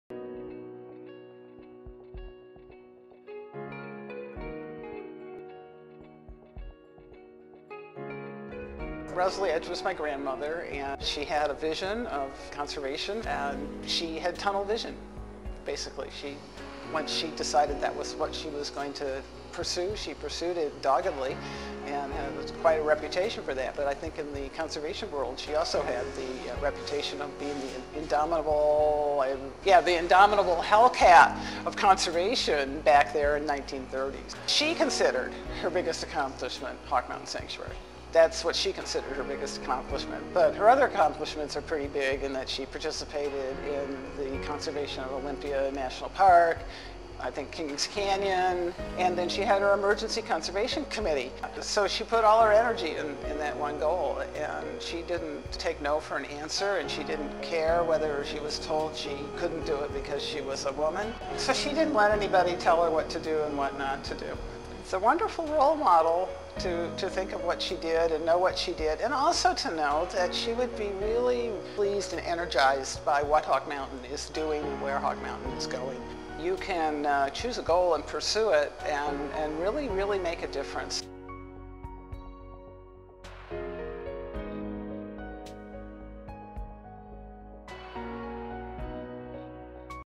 Amazing Role Model for Women Worldwide: The Triumph of Rosalie Edge
Hawk Mountain Sanctuary’s founder Rosalie Edge is an excellent role model for women and conservationists alike. In this video, her granddaughter Deborah Edge describes Rosalie’s accomplishments and how she impacted raptor conservation and women’s history.